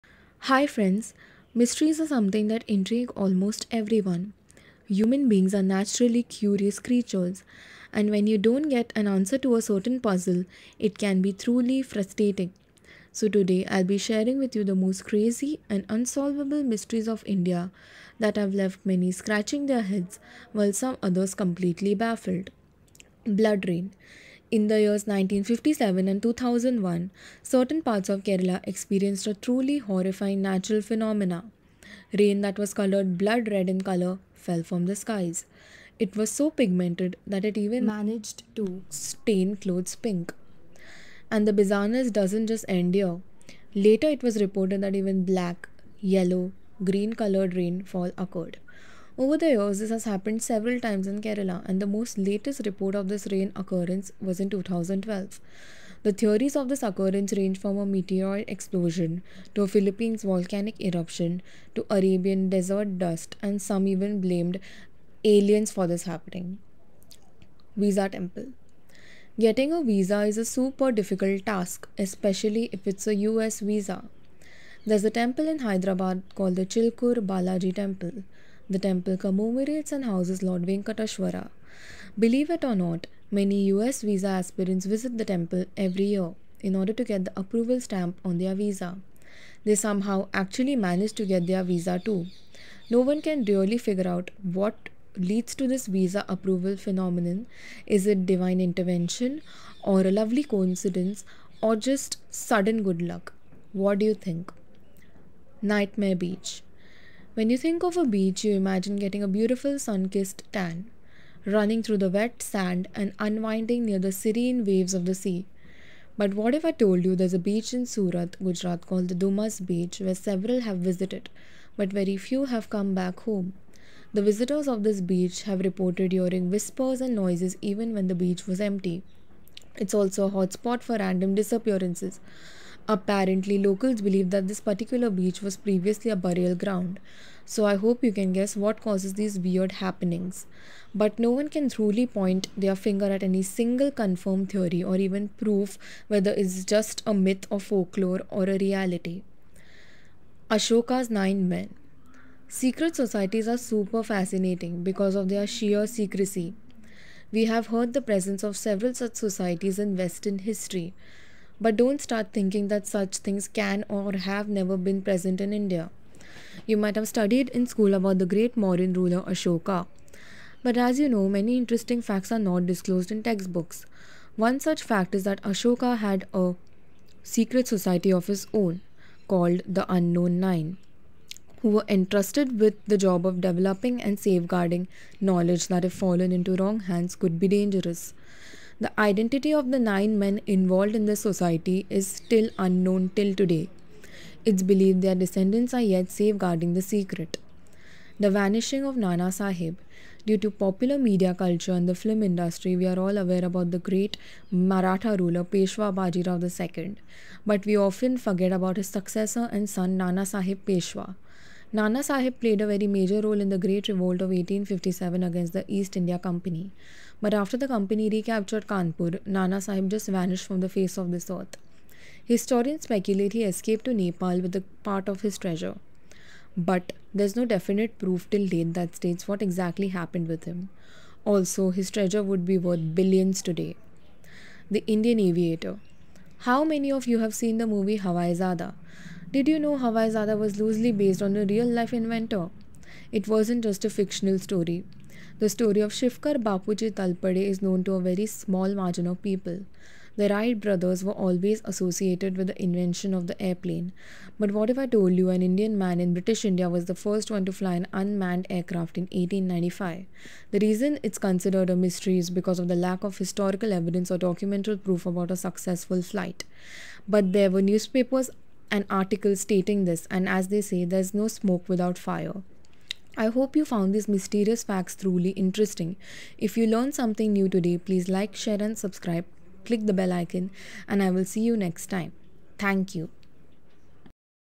Unsolved mysteries of India
When questions remain unanswered and mysteries remain unsolved for a long period of time, it yet causes a lot of curiosity. Watch the video to be enlightened with the most mysterious stories from India and also put on your thinking caps to guess, what might have been the true reality behind these stories.

Image credit- Shutterstock, google images